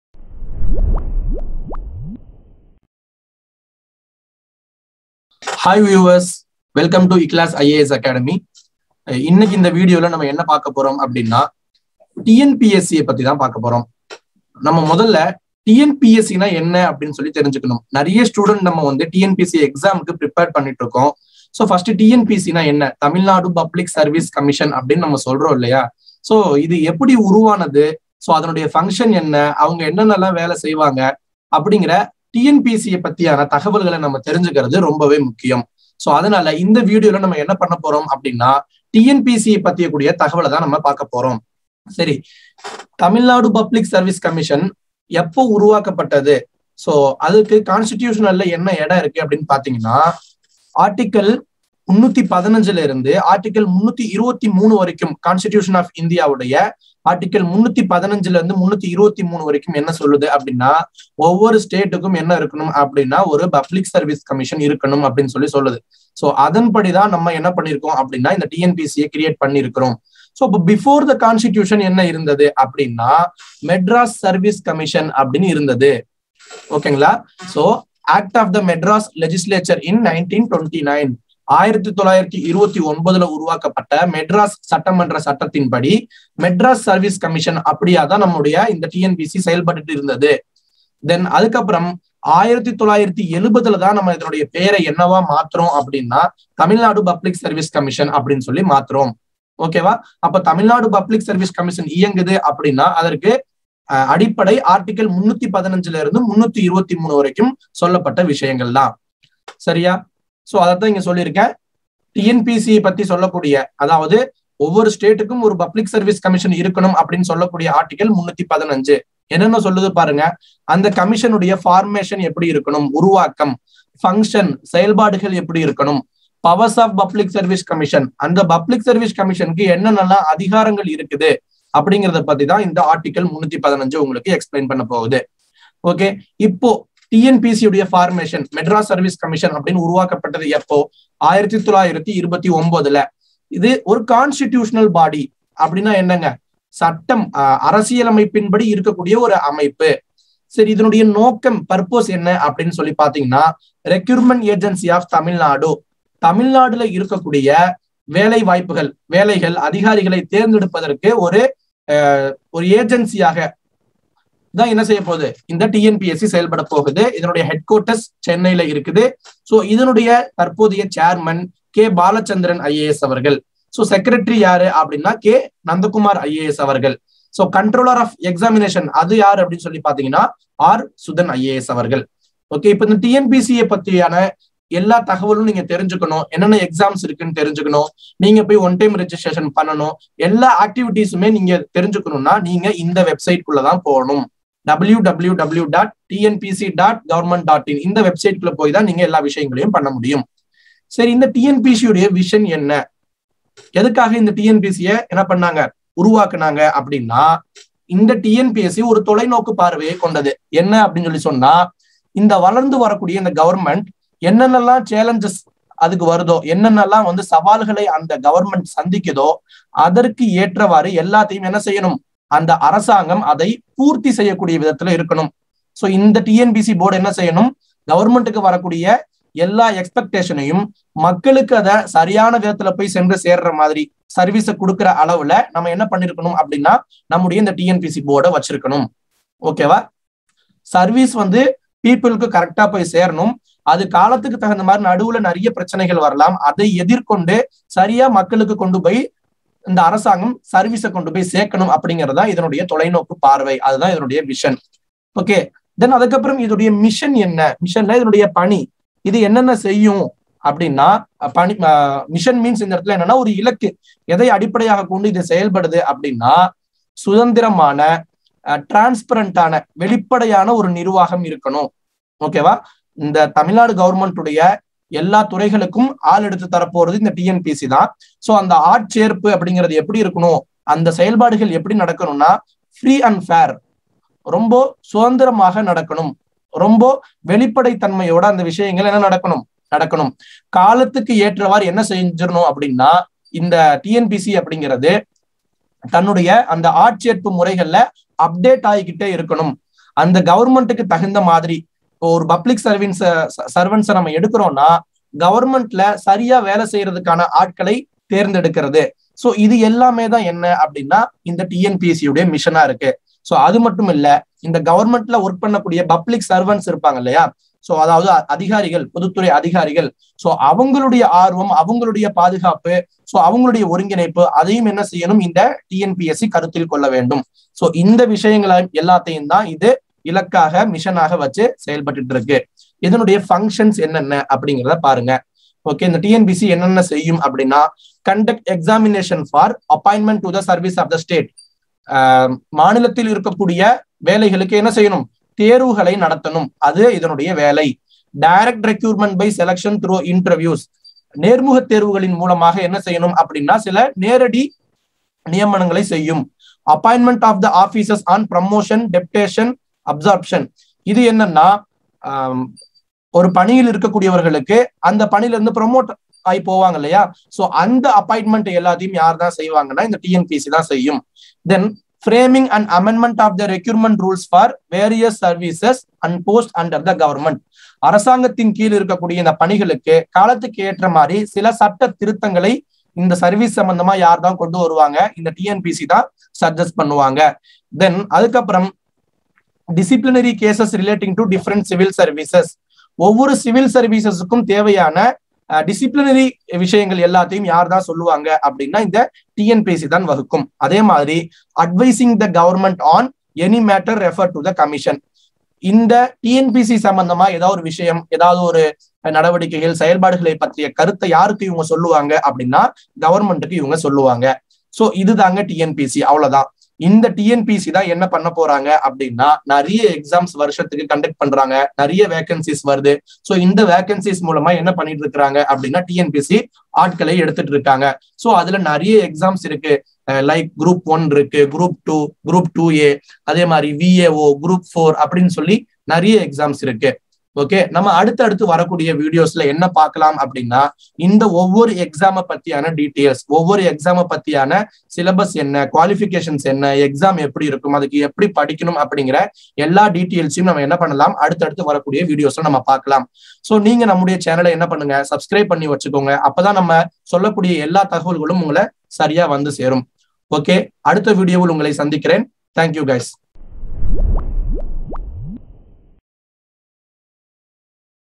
TNPSC என்றால் என்ன? | TNPSC யும் அதன் பணிகளும்.
This video is about TNPSC and its functions